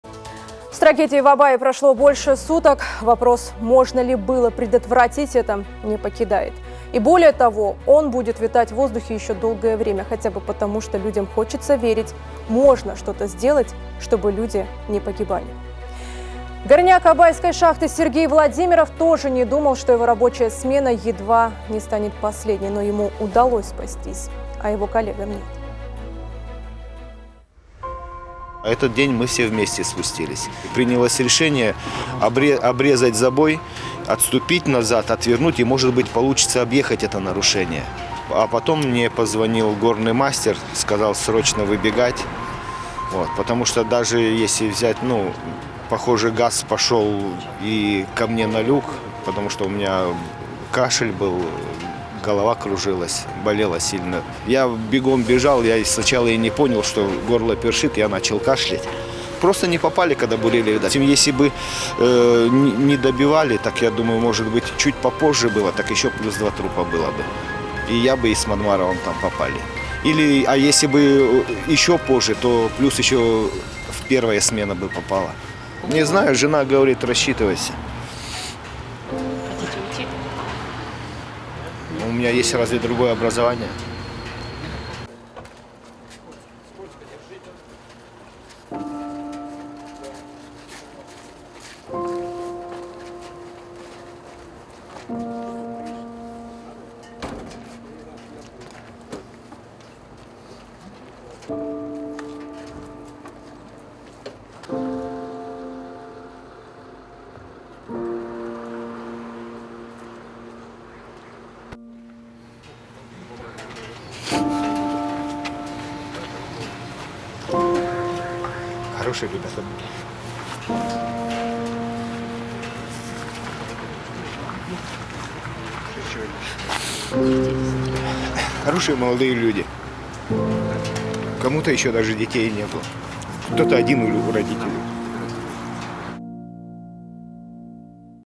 История выжившего на шахте «Абайская» в Караганде
Накануне в Абае из-за аварии на шахте погибли шесть горняков. Причина - выброс метана. Самому старшему было 53 года, младшему - 30 лет. Почти у всех остались семьи. Двое рабочих остались живы, но получили травмы.
Горняк Абайской шахты Сергей Владимиров даже не думал, что его рабочая смена едва не станет последней.
В роковой день спустился с коллегами как обычно. Через время мужчине позвонили и приказали срочно выбегать. Ему удалось спастись. А коллегам нет.

Во всём информационном потоке мы выбираем то, о чём действительно стоит задуматься. С нами Всё станет понятно